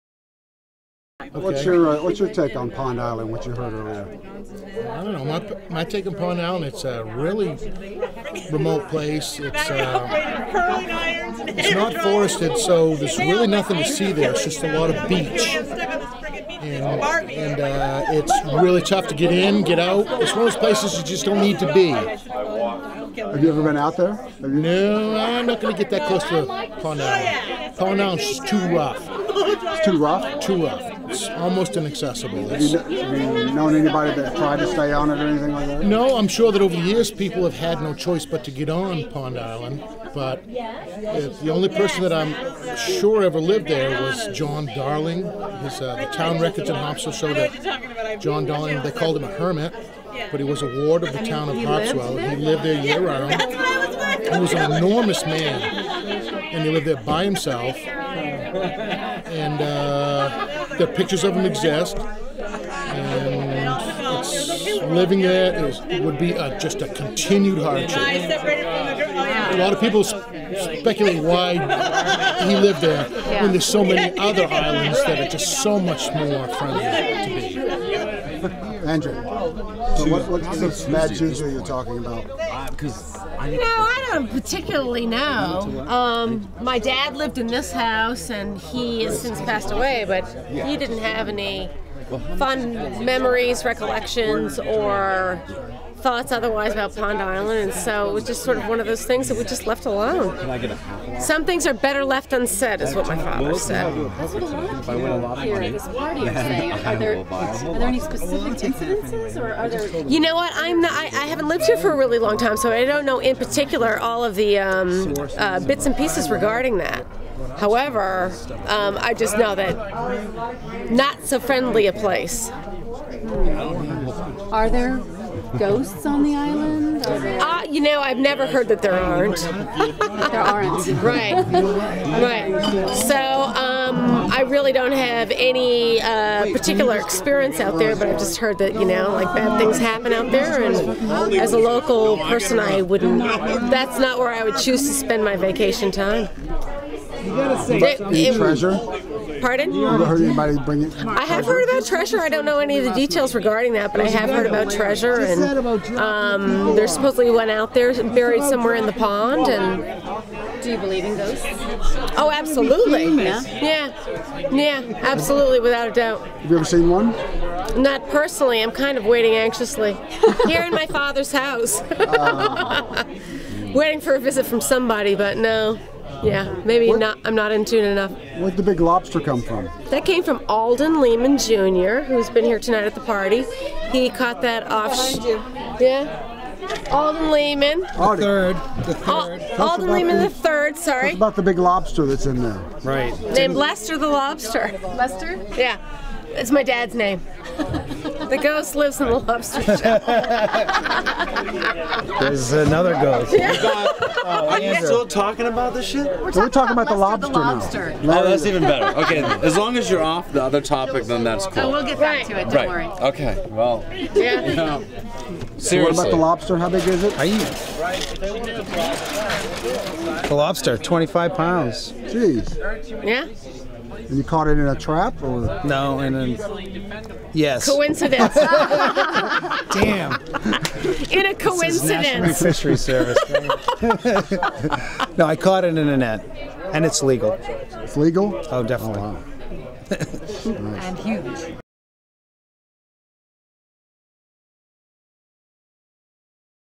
Pond Island: Locals share their opinion - Part II
Locals Aldie, Andrea and Joann share what they know about Pond Island.